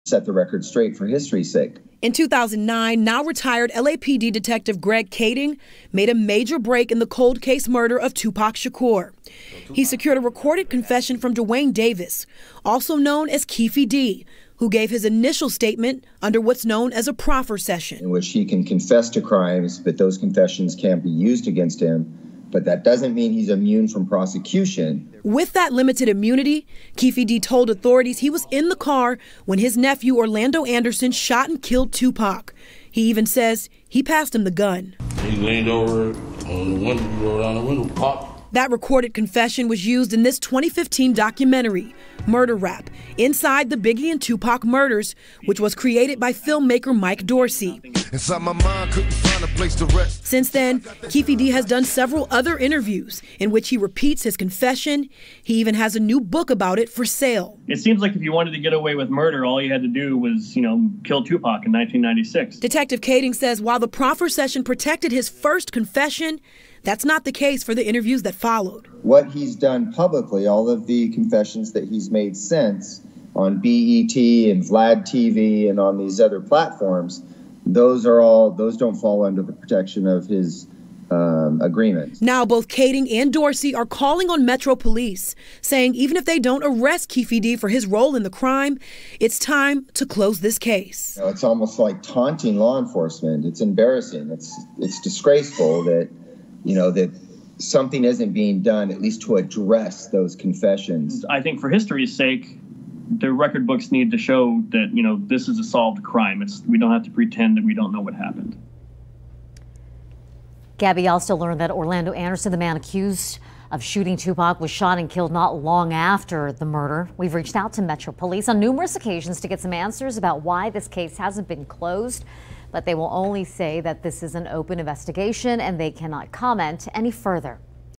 Documented confessions result in calls for LVMPD to close Tupac Shakur murder case
Rapper Tupac Shakur was shot and killed in Las Vegas 23-years-ago.

In 2009, now retired LAPD Detective Greg Kading made a major break in the cold case murder.

Kading secured a recorded confession from Duane Davis, also known by his street name Keffe D. He gave his initial statement under what's known as a proffer session.